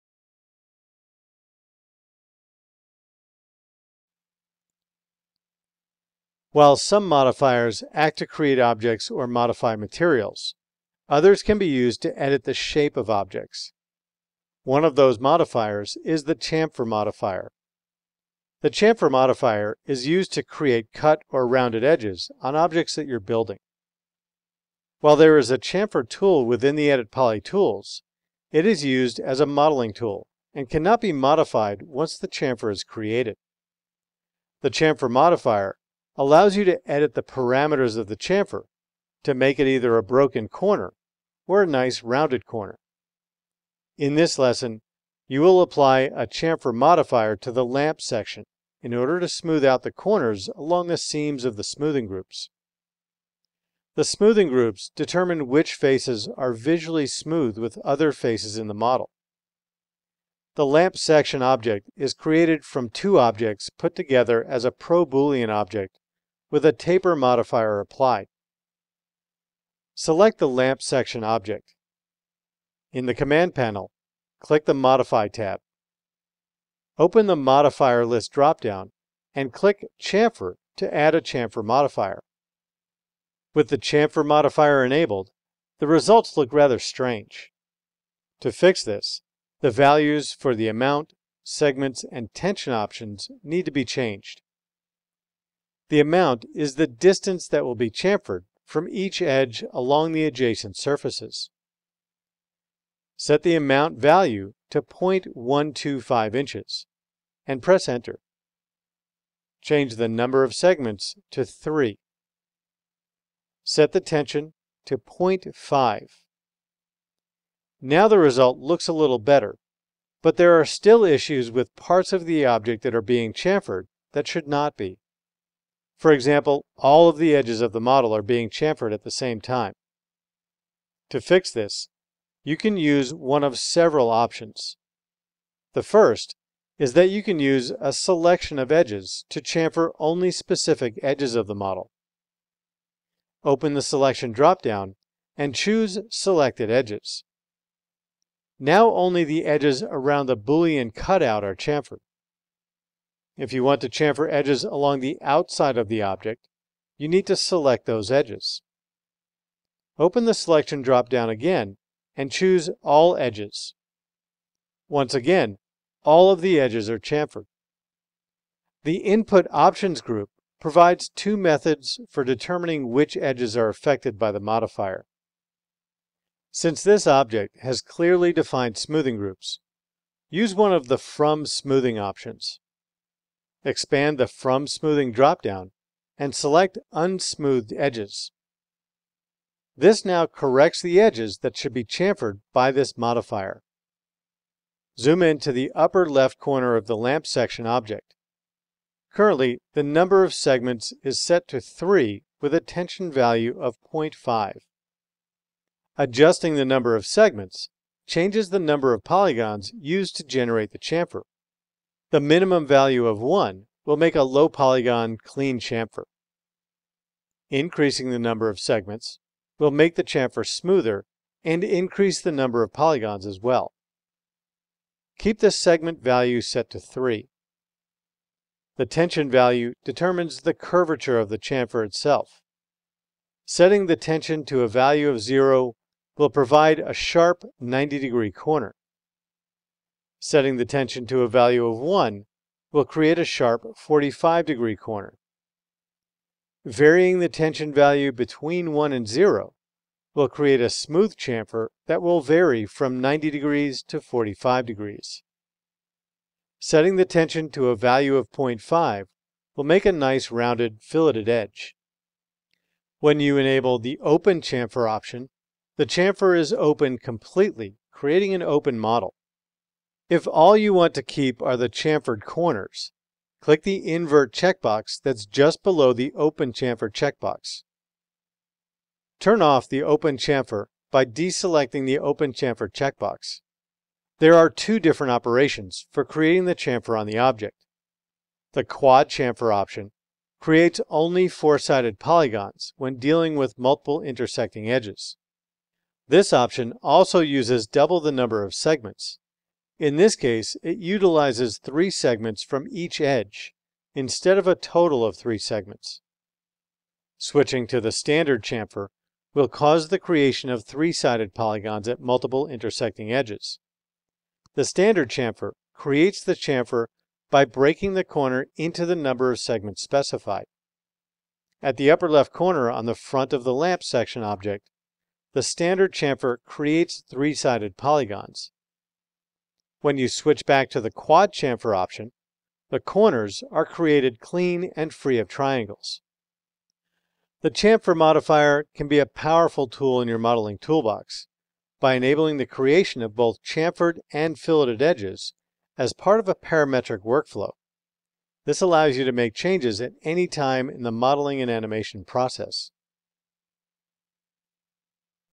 Working with the Chamfer Modifier
How to use the Quad Chamfer modifier to model a mechanical model in Autodesk 3ds Max 2015.